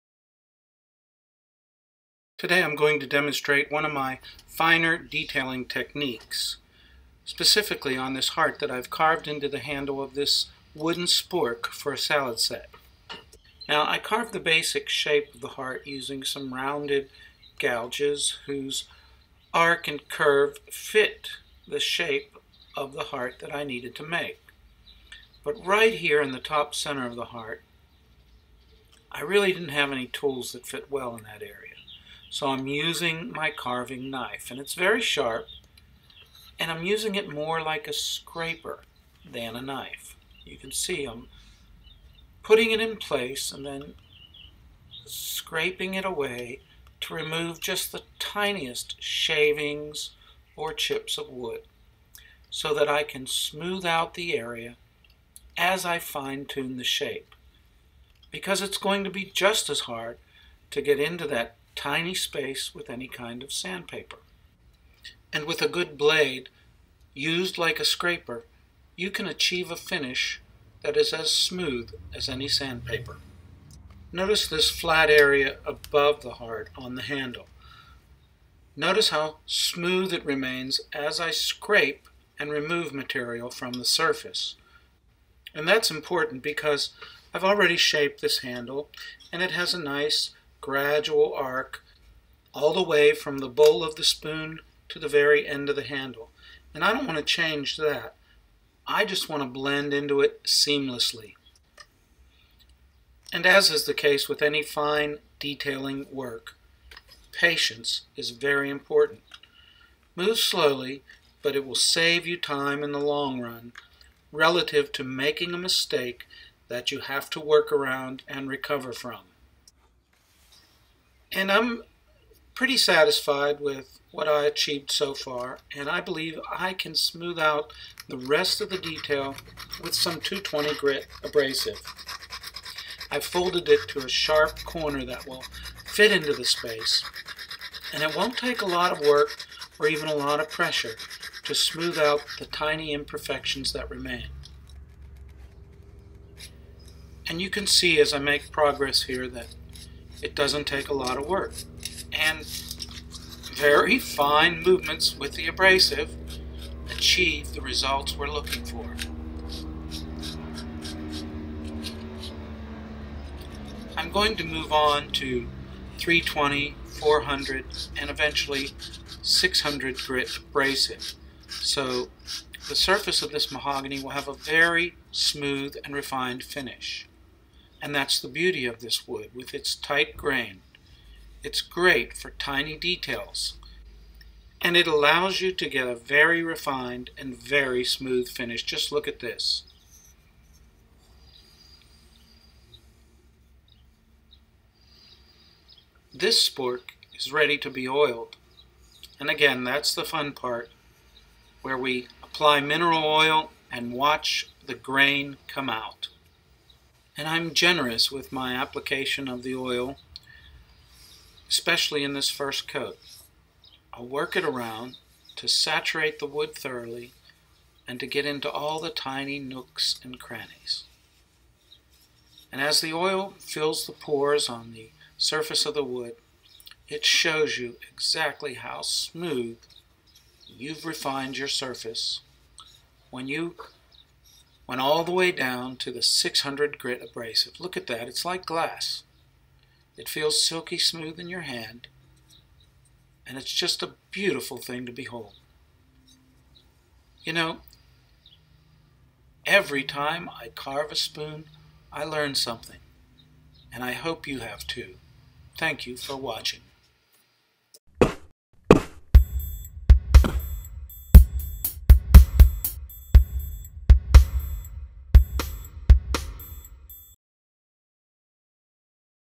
Fine Detailing A Hand Carved Spork
Using my carving knife like a scraper in the tight corners where I have no other tools that fit.